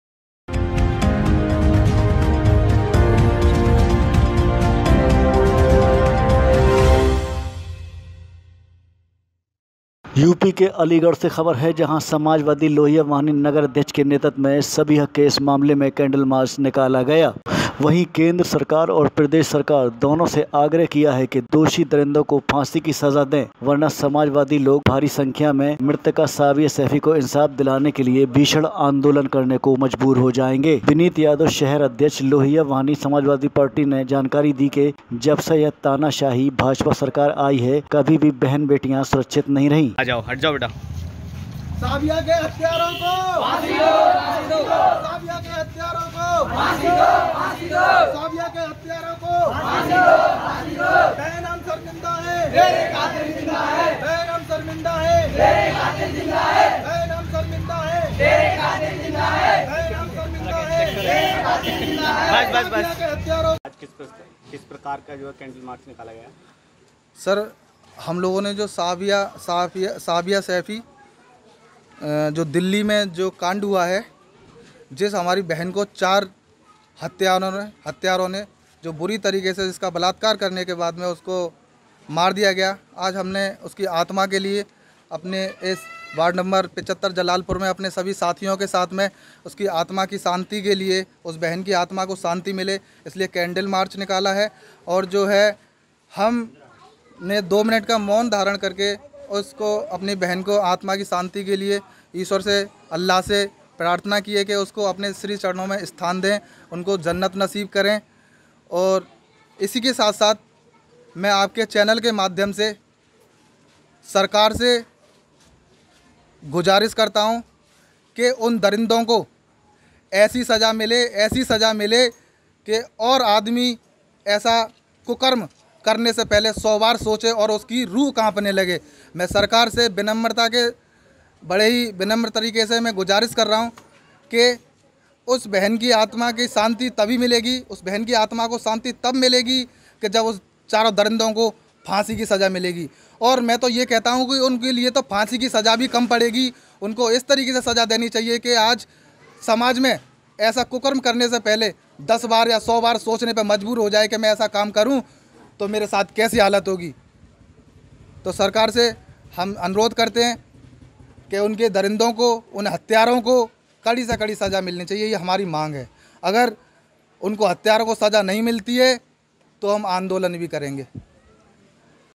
अलीगढ़ : सपा लोहिया वाहिनी नगर अध्यक्ष के नेतृत्व में सबीहा केस मामले में निकाला कैंडल मार्च।
देहली में बहन साबिया सैफ़ी के साथ हुई बलात्कार की गंभीर घटना को लेकर और बहन की आत्मा शांति के लिए 2 मिन्ट्स का मौन धारण कर आत्मा की शांति के लिए ईश्वर से प्रार्थना की और वही केंद्र सरकार और प्रदेश सरकार दोनों से आग्रह किया कि दोषी दरिंदो के फाँसी की सजा दो वार्ना हम समाजवादी लोग भारी संख्या में बहन साबिया सैफ़ी को इंसाफ़ दिलाने के लिए भीषण आंदोलन करने को मजबूर हो जाएंगे ।